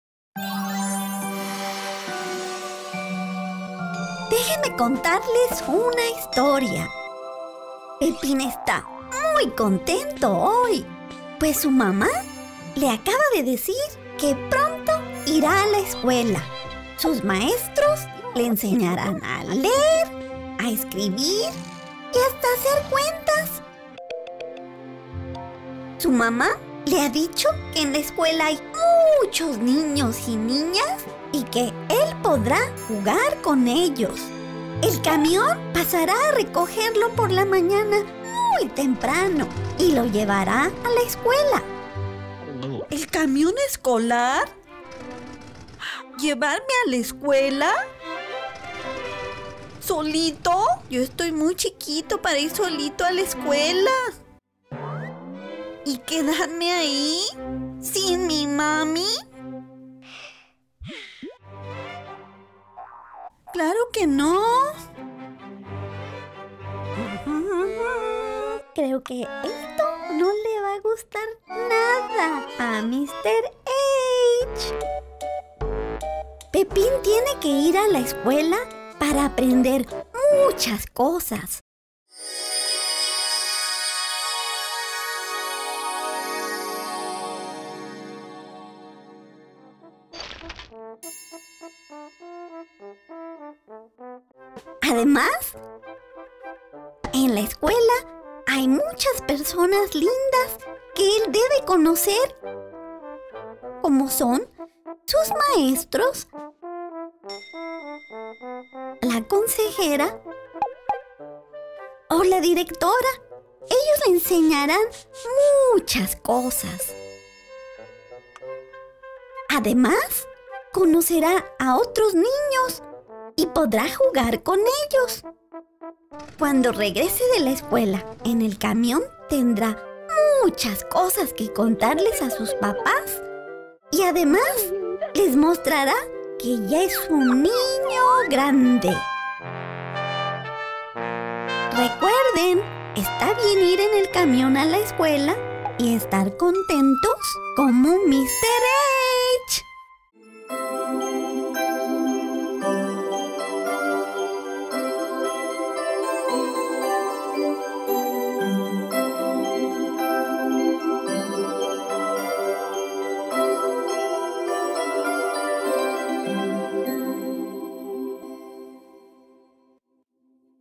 El primer día de escuela
Se presenta a un niño llamado Pepín pensando en el primer día de escuela. De forma divertida y educativa. El encuentra soluciones para disfrutar esa experiencia.